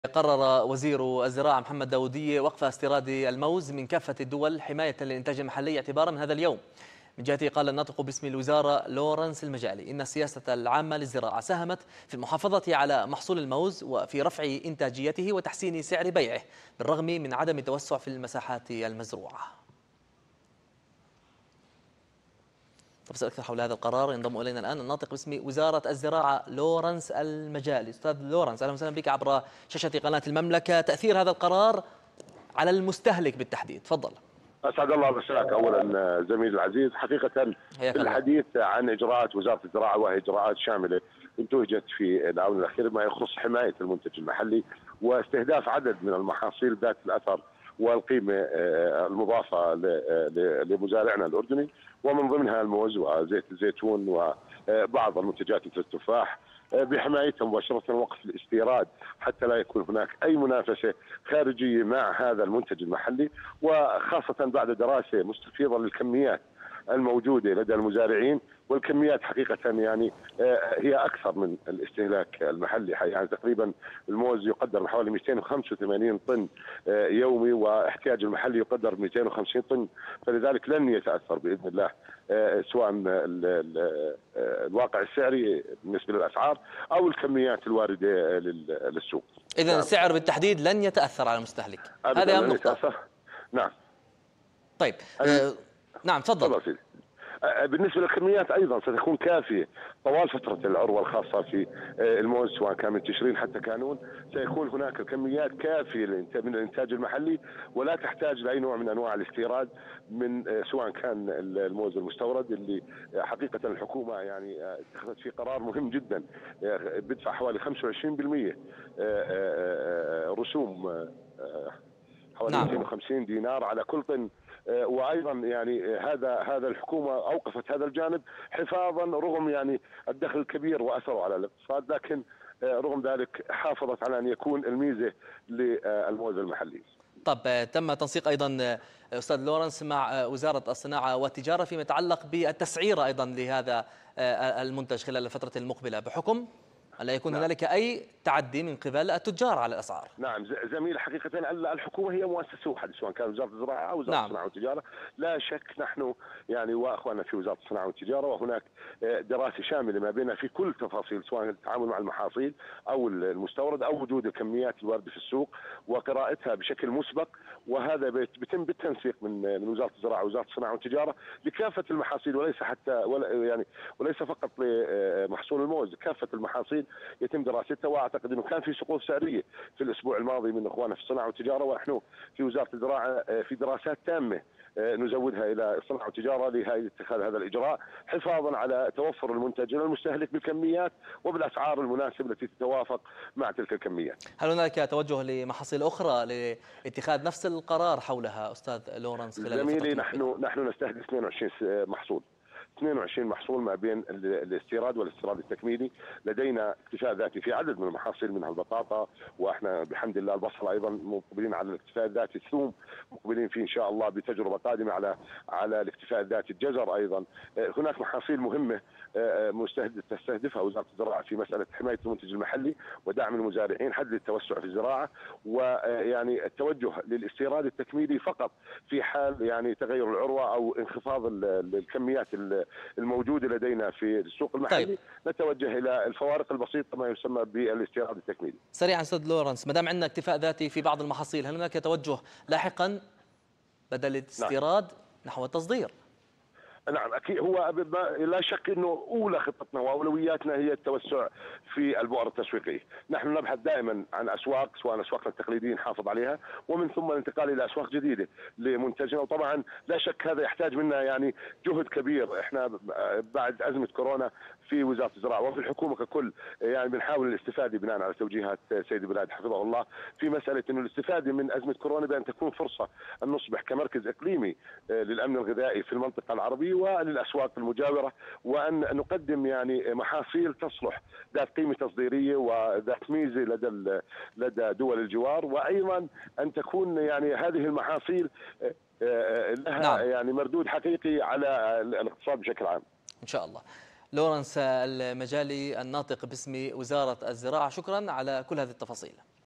الناطق باسم وزارة الزراعة: قرار وقف استيراد الموز لن يؤثر على المستهلك أو الأسعار
مقابلة  الناطق باسم وزارة الزراعة، لورنس المجالي

هنا_المملكة